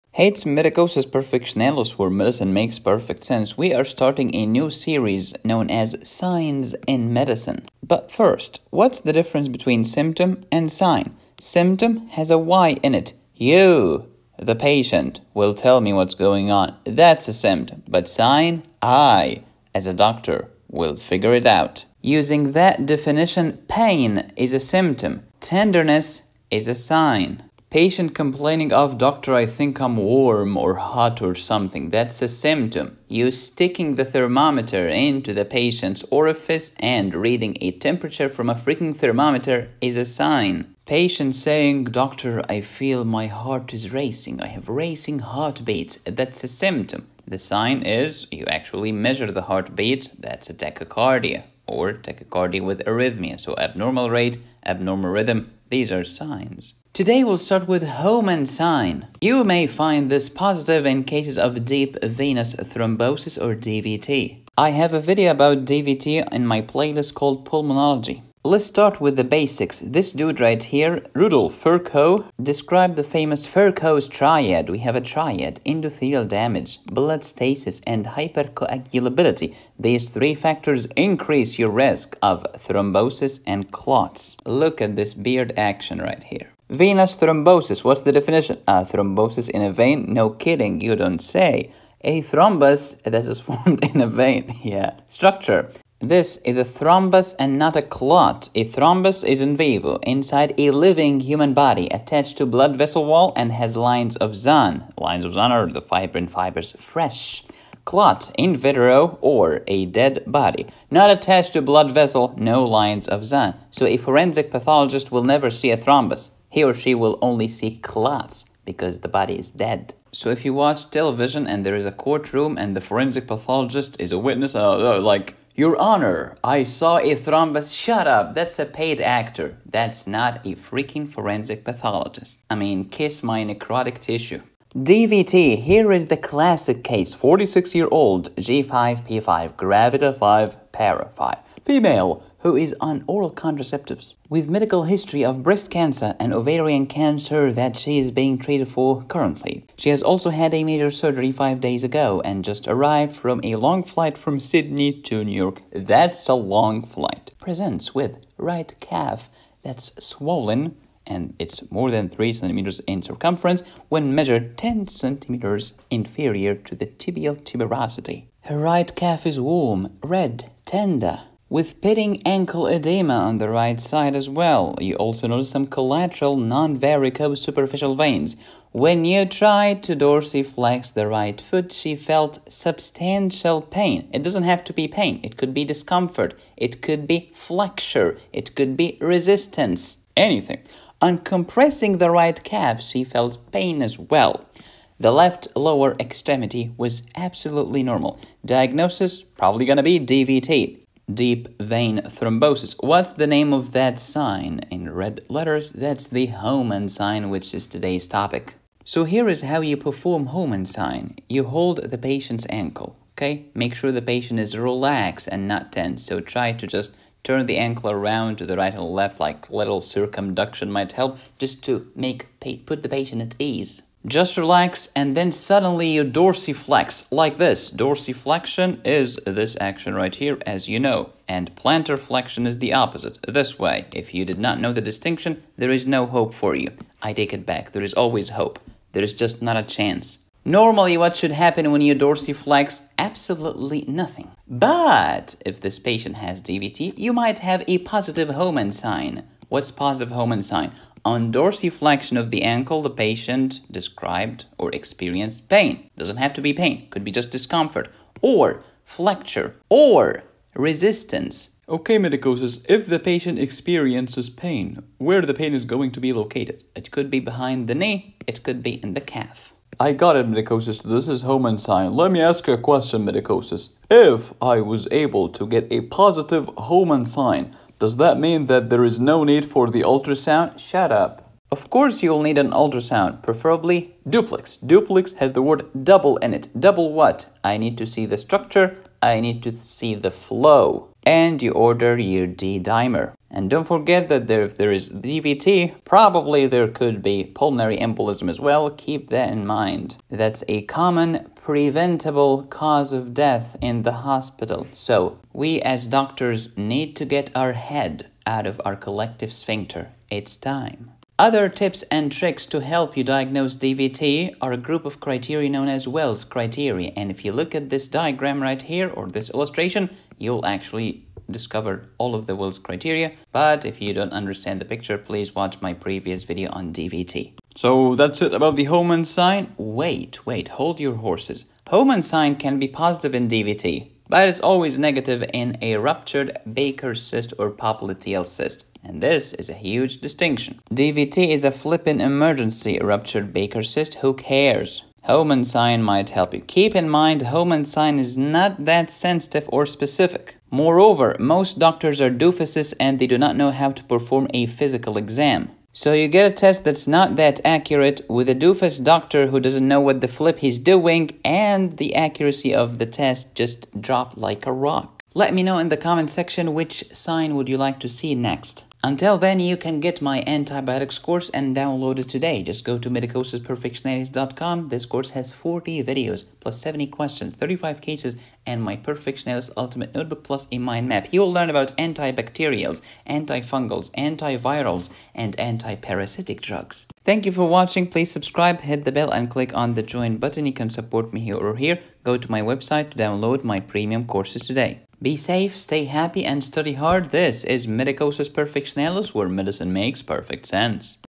Homans Sign - Deep Vein Thrombosis (DVT) - Internal Medicine - Physical Exam
Homan Sign | Internal Medicine..

Happy studying!!

📊 My favorite Productivity Apps 📱Save on your mobile phone bill 🏦 Qbank (TrueLearn)